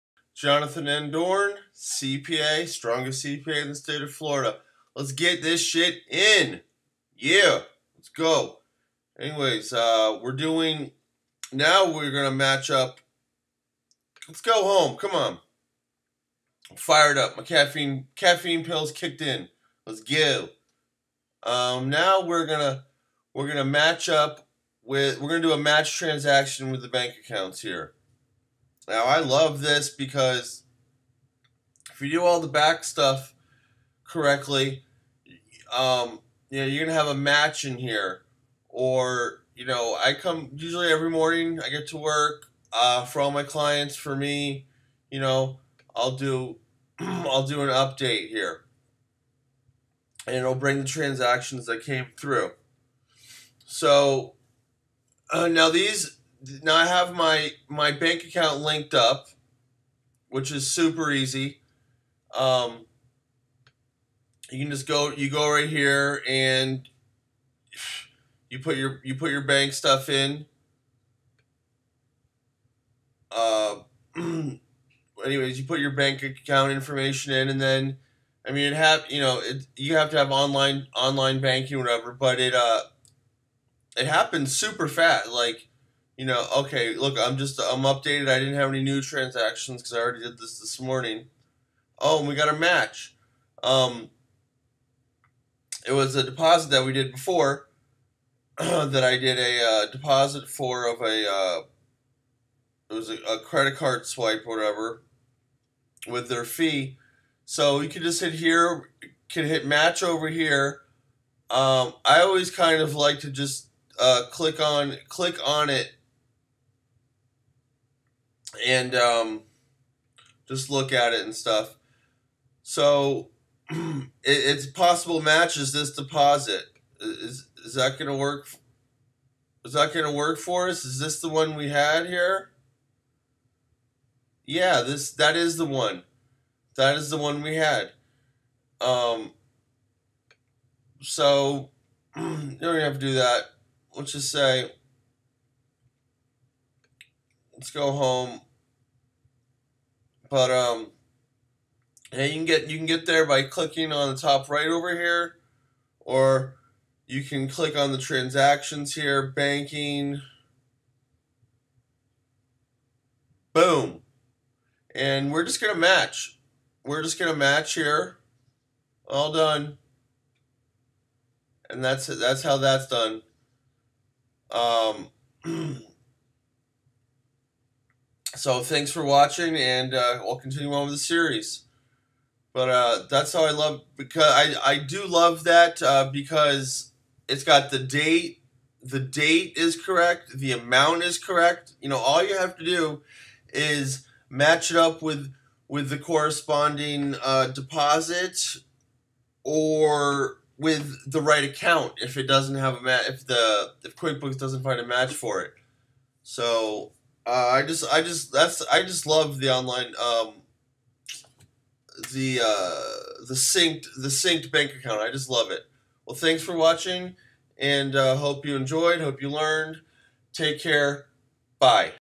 QuickBooks Online Tutorial for beginners / Matching deposits with synced Bank Accounts #5
This a QuickBooks Online Tutorial for beginners.  I love QuickBooks Online and use it for my business as well as Multiple clients businesses.In this QuickBooks Tutorial series i go over making an estimate and converting it to an invoice, sales receipts,  deposits with credit card fees, matching deposits with synced bank accounts, customized Reports and so much more.